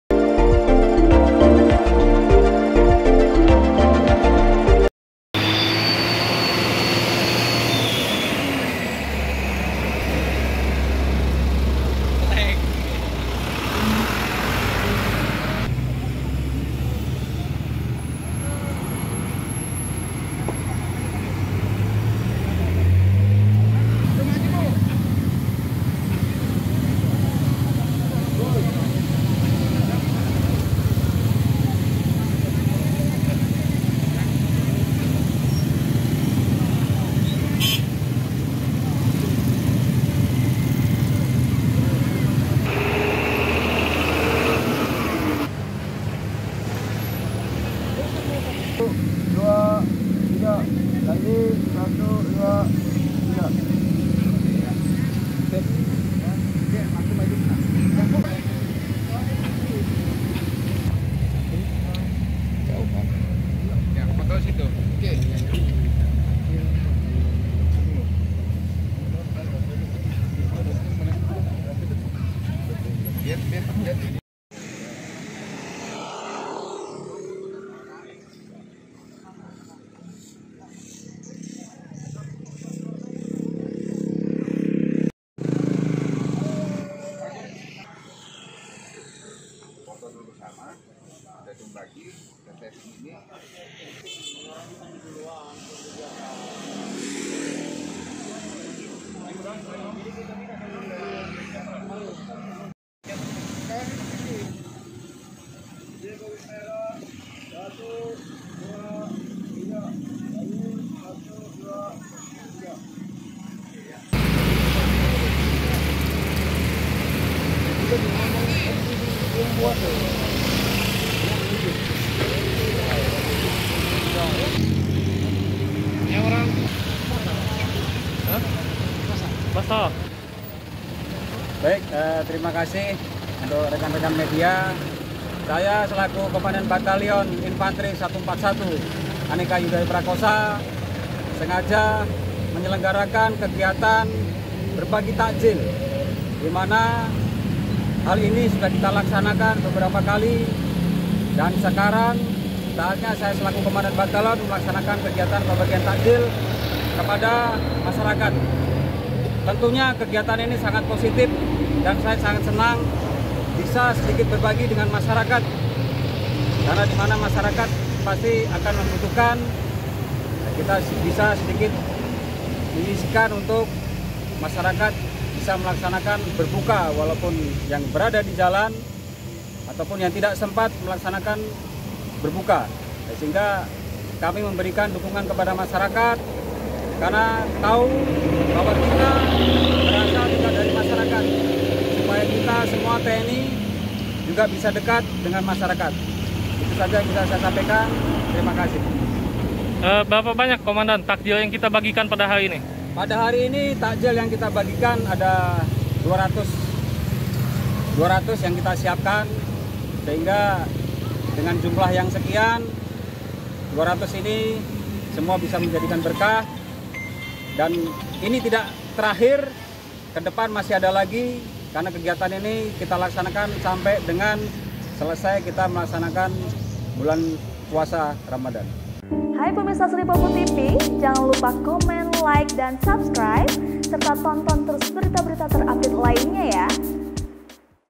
Batalyon Infanteri 141 Muara Enim Bagi Bagi Takjil Gratis
SRIPOKU.COM, MUARA ENIM- Dalam rangka kegiatan Berkah Ramadhan, Yonif 141/AYJP Muara Enim membagikan 200 Takjil secara gratis kepada masyarakat dan pengguna jalan.
Kegiatan tersebut dilakukan di depan Mako Yonif 141/AYJP Muara Enim dan SDN 18 Muara Enim, Kabupaten Muara Enim, Jumat (22/4/2022).

Danyonif 141/AYJP Muara Enim Letkol Inf Dani Indrajaya bersama jajaran perwira dan anggota dengan telaten membagikan Takjil kepada masyarakat yang melintas terutama yang menggunakan kendaraan untuk berbuka puasa. Meski sempat diguyur hujan namun tidak menyurutkan anggota Yonif 141/AYJP membagikan takjil Ingga ludes.

Lihat informasi terkini bersama kami, di:

Palembang.tribunnews.com

Sumber Video:Ardani Zuhri
Editor Video:Tarso